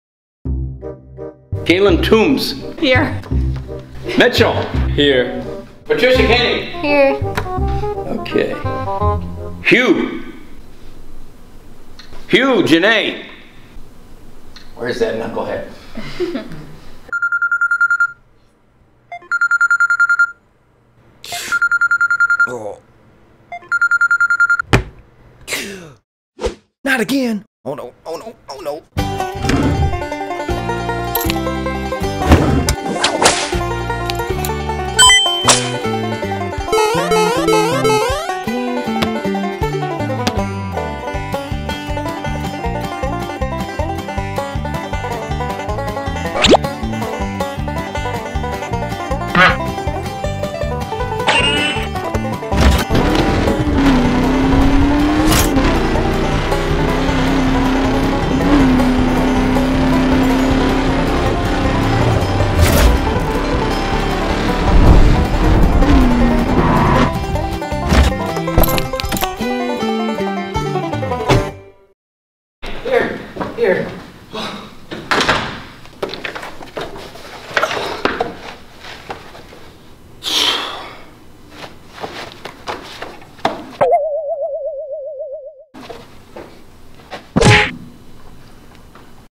Late For College!!
A sequel to Late For School, Hugh to get to his class at college but he got up to late.